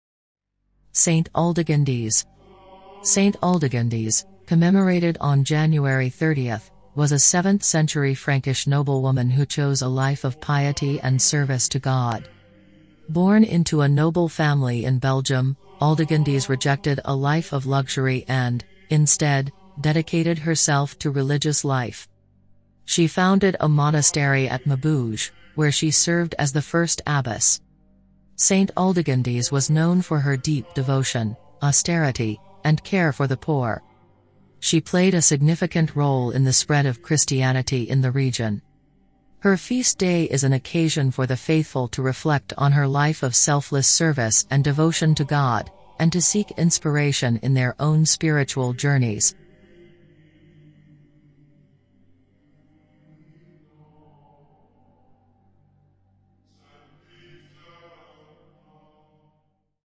🌟 Saint Aldegundis: Radiant Light of Compassion and Healing | SaintAldegundis CatholicSaints 🌟
Step into the luminous world of Saint Aldegundis, a beacon of compassion and healing in the Catholic tradition. 🌟 Discover the remarkable life of this saint, known for her unwavering dedication to caring for the sick and the marginalized. In this exploration, we delve into the miracles associated with Saint Aldegundis and reflect on the timeless lessons her life offers to contemporary believers. Join us in celebrating the legacy of Saint Aldegundis, whose compassionate spirit continues to inspire acts of kindness and mercy today. 🙏✨

🌺 Key Highlights:

Life of Saint Aldegundis: Uncover the inspiring story of Saint Aldegundis, her early life, her call to religious life, and her profound impact on the community.

Compassion and Healing Miracles: Explore the miracles attributed to Saint Aldegundis, particularly those related to healing and acts of compassion. Witness the divine interventions that showcase her special connection to the healing grace of God.

Lessons for Today: Reflect on the timeless lessons from Saint Aldegundis' life, drawing inspiration for acts of compassion, mercy, and service in our contemporary lives.

Prayerful Reflection: Engage in a prayerful reflection, seeking Saint Aldegundis' intercession for healing, compassion, and strength in times of need.

✨ Saint Aldegundis' Light Today:
Immerse yourself in the radiant light of Saint Aldegundis and be inspired to carry her legacy forward by embracing compassion, healing, and mercy in your own journey of faith. 🌟🙌